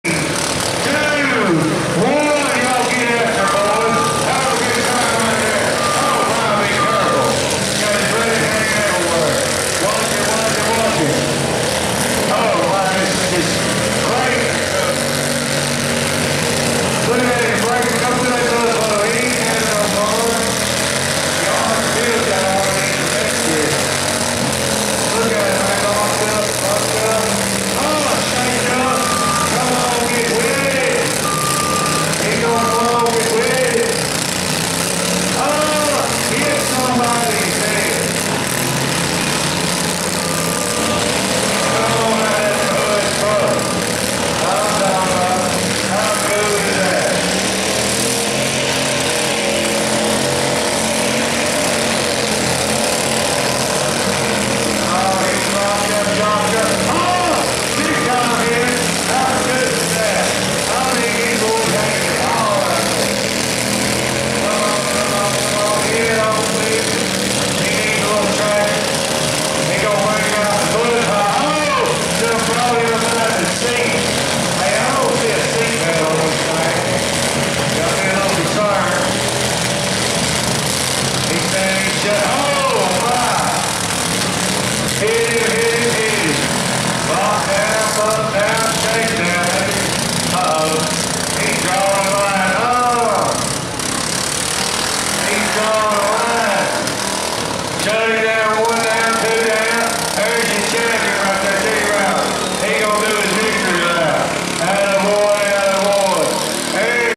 Robertson Co. Spring Derby 2019 Mowers
The lawnmower heat from the Robertson County Spring Derby.
Derby Held in Springfield, TN